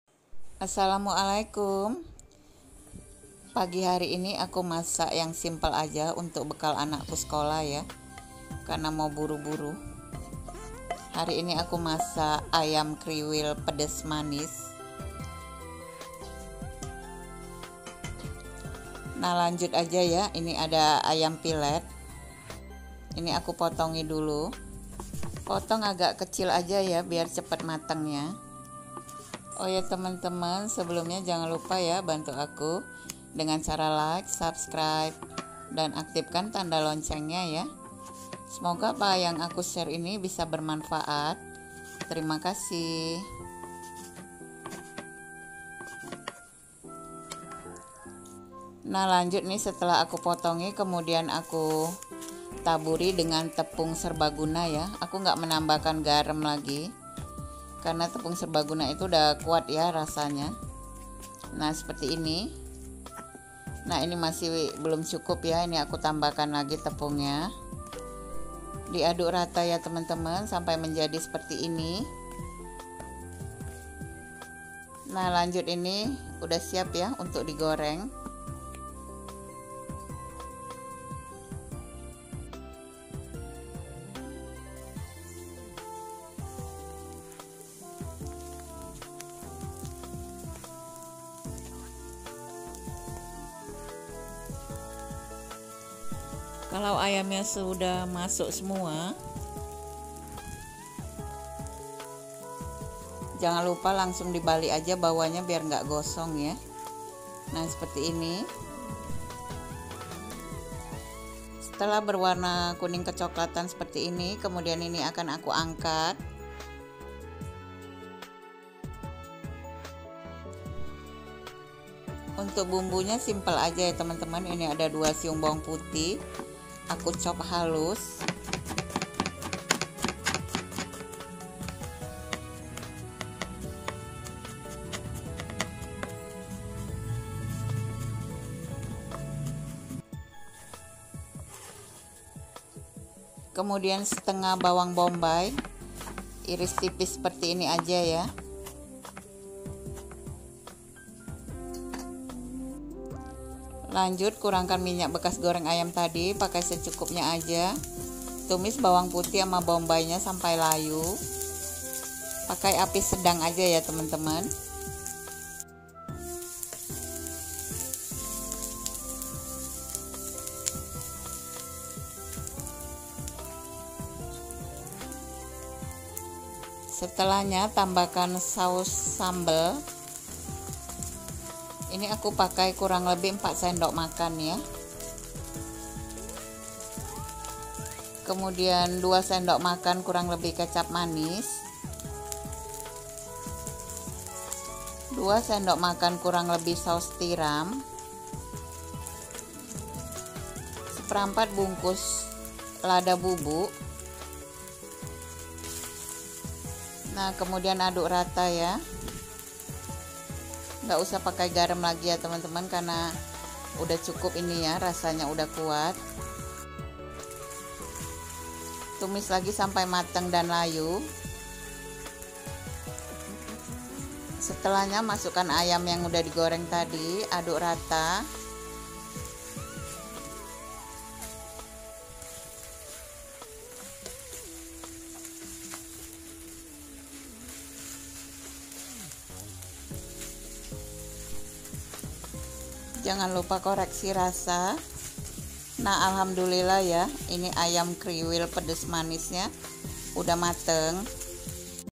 Bekal Simple || Ayam Keriwil Pedas Manis
Jadi menu dan resep yang saya hadirkan hari ini adalah ayam keriwil pedas manis, saya harap video saya buat dapat menambah pilahan menu makanan sehari-hari dirumah. ________________  ^_^ ^_^ ^_^